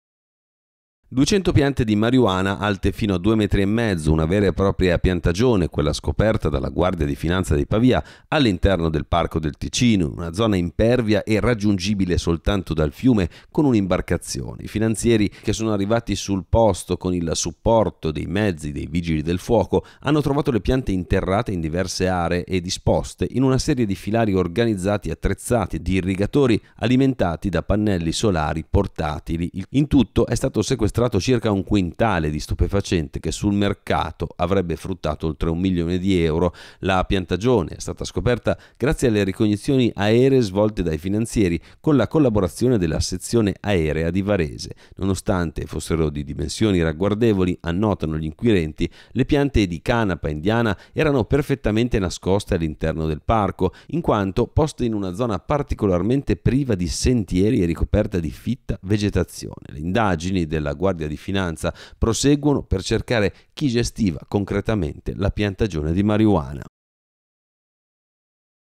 Parco del Ticino, la Guardia di Finanza scopre e sequestra una piantagione di marijuana
200 piante di marijuana alte fino a 2 metri e mezzo: una vera e propria piantagione quella scoperta dalla Guardia di finanza di Pavia nel Parco del Ticino, in una zona impervia e raggiungibile solo dal fiume con un imbarcazione. I finanzieri, che hanno raggiunto il posto con il supporto dei mezzi dei vigili del fuoco - hanno trovato le piante interrate in diversi aree e disposte in una serie di filari organizzati e attrezzati di irrigatori alimentati da pannelli solari portatili. In tutto è stato sequestrato circa un quintale di stupefacente che sul mercato avrebbe fruttato oltre un milione di euro. La piantagione è stata scoperta grazie alle ricognizioni aeree svolte dai finanzieri con la collaborazione della sezione Aerea di Varese. Nonostante fossero di dimensioni ragguardevoli - annotano gli inquirenti - le piante di canapa indiana - erano “perfettamente nascoste all'interno del Parco, in quanto poste in una zona particolarmente priva di sentieri e ricoperta da fitta vegetazione”.  Le indagini della Guardia di Finanza proseguono per cercare chi gestiva concretamente la piantagione.

(00:01) 200 piante di marihuana alte fino a 2,5 m una vera e propria piantagione quella scoperta dalla guardia di finanza di Pavia all'interno del parco del Ticino una zona impervia e raggiungibile soltanto dal fiume con un'imbarcazione I finanzieri che sono arrivati sul posto con il supporto dei mezzi dei Vigili del Fuoco hanno trovato le piante interrate in diverse aree e disposte in una serie di filari organizzati attrezzati di irrigatori alimentati da pannelli solari portatili in è stato sequestrato circa
(00:31) un quintale di stupefacente che sul mercato avrebbe fruttato oltre un milione di euro la piantagione è stata scoperta grazie alle ricognizioni aeree svolte dai finanzieri con la collaborazione della sezione aerea di Varese nonostante fossero di dimensioni ragguardevoli annotano gli inquirenti le piante di canapa indiana erano perfettamente nascoste all'interno del parco in quanto posta in una zona particolarmente priva di Sentieri e ricoperta di fitta vegetazione Guardia di Finanza proseguono per cercare chi gestiva concretamente la
(01:05) piantagione di marijuana